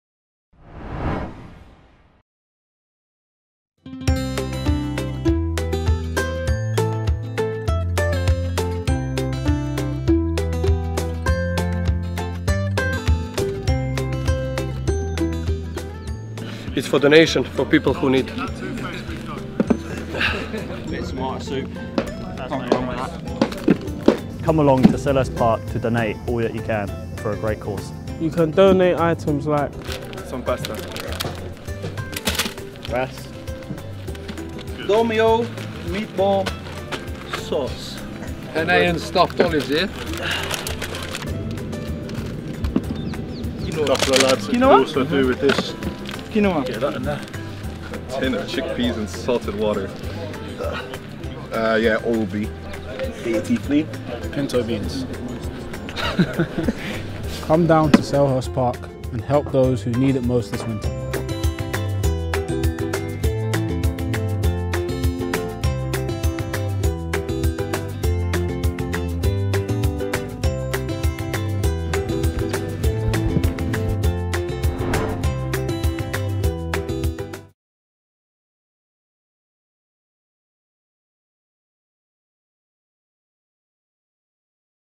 Support the Palace Christmas foodbank drive | Palace For Life
We have teamed up with City Harvest and Norwood & Brixton FoodBank to provide food to those who need it most this winter 🤝